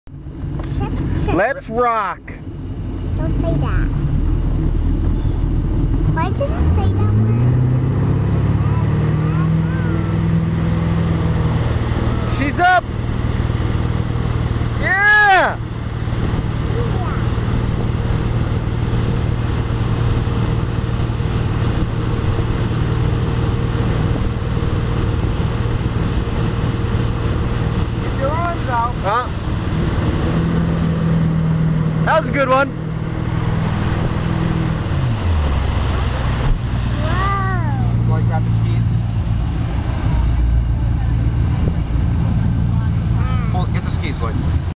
My second attempt...
I tried again but I wiped out.  I totally hurt myself this time around.  He called me a wimp but whatever, I was done for the day.